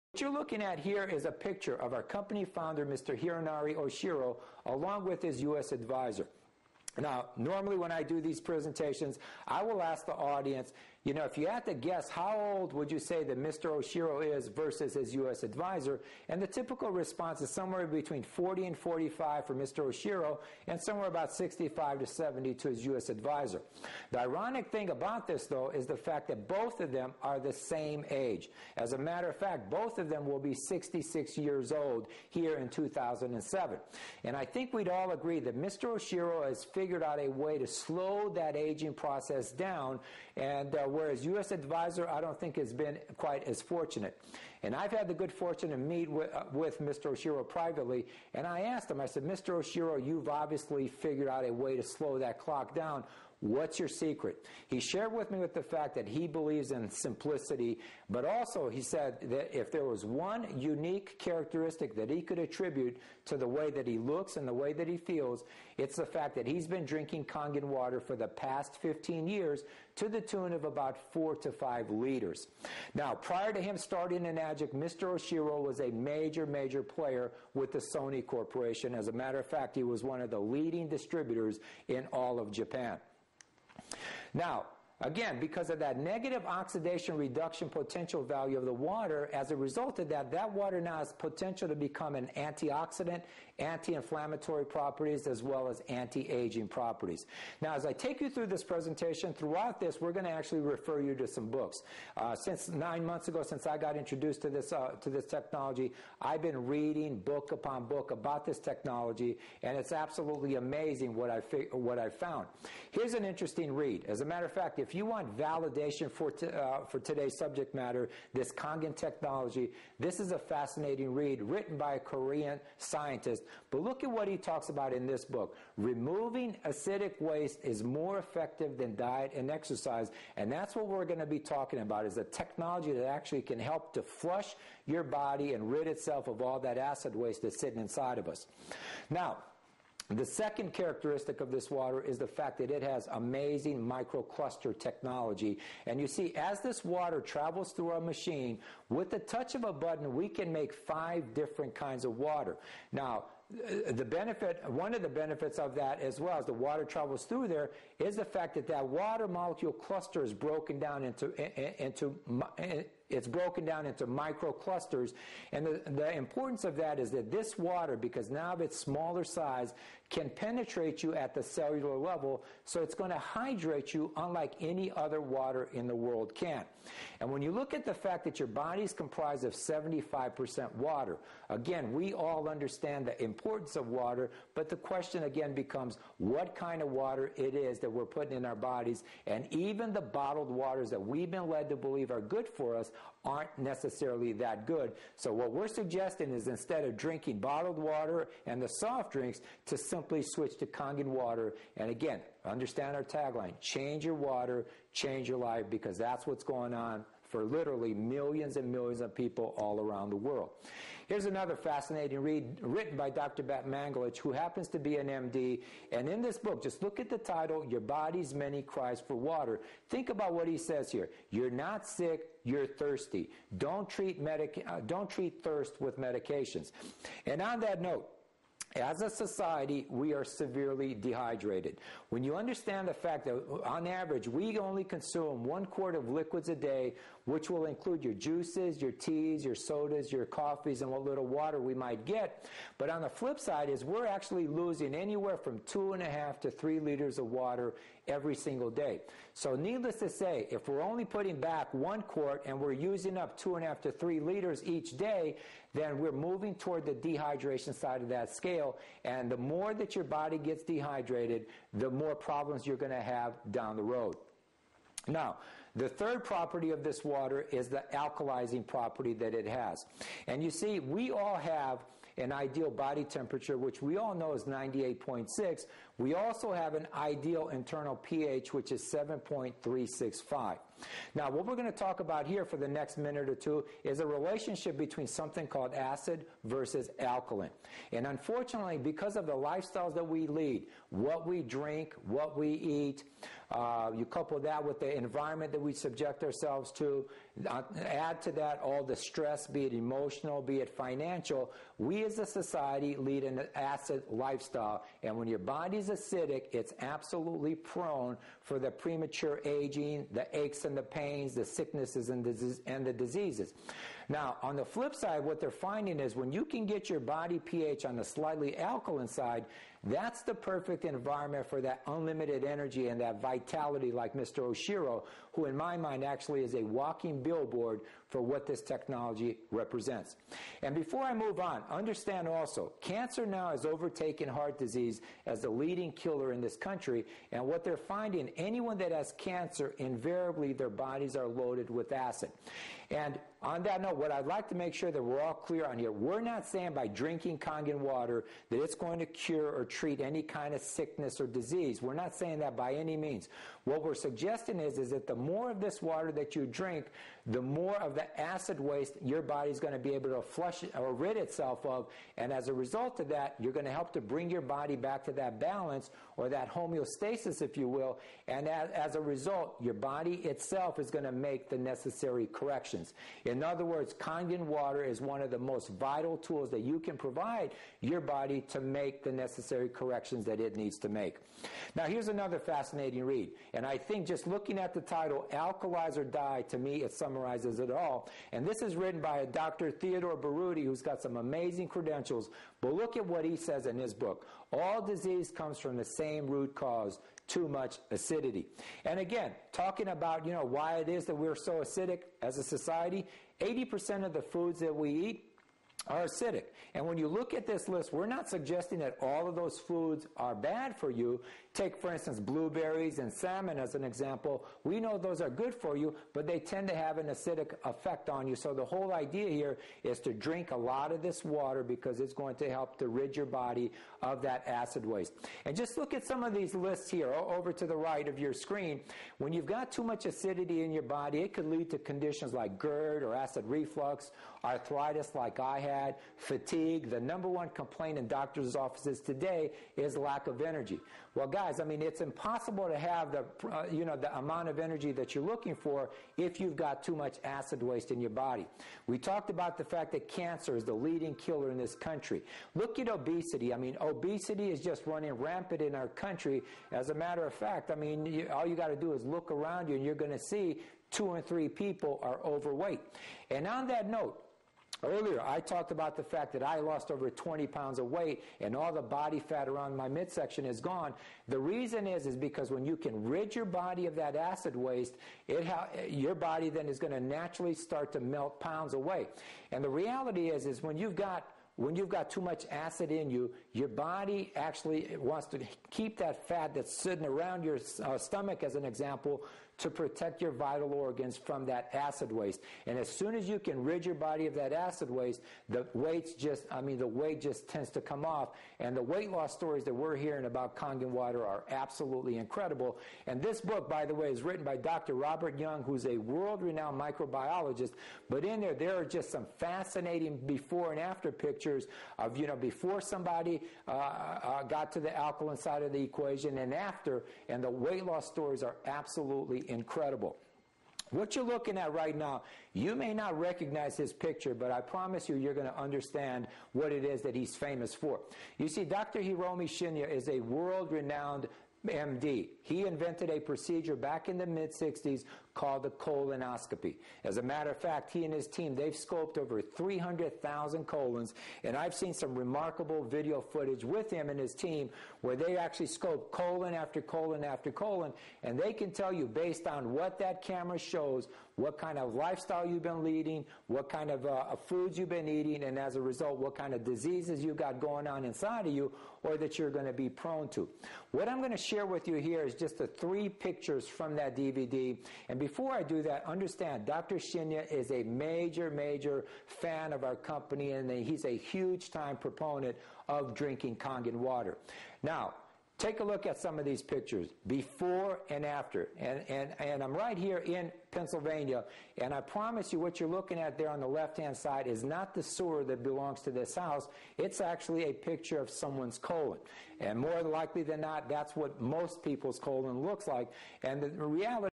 Health Revolution 2
75% of the Human Body is made up of water. What water are you made of? Contaminated city water, unpurified well water, sterilized bottle water (also reverse osmosis) or perhaps you will soon be drinking Kangen water.